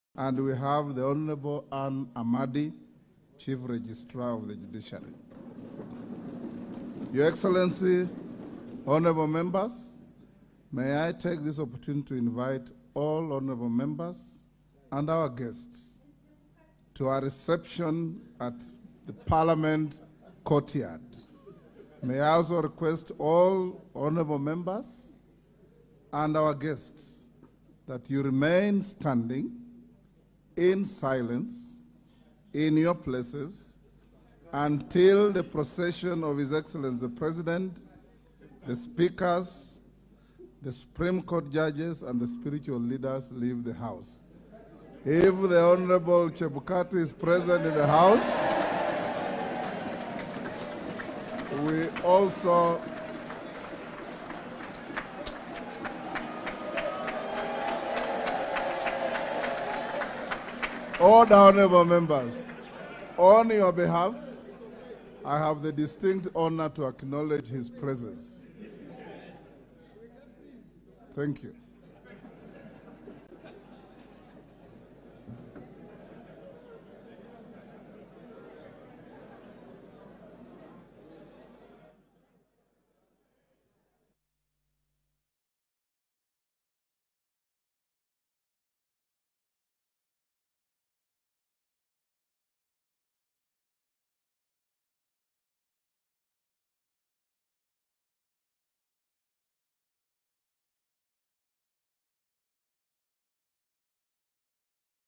LOWKEY BOOS AS WETANGULA RECOGNISES THE PRESENCE OF CHEBUKATI.
Lowkey boos as Wetangula recognises the presence of Chebukati during the openiing of the 13TH parliament.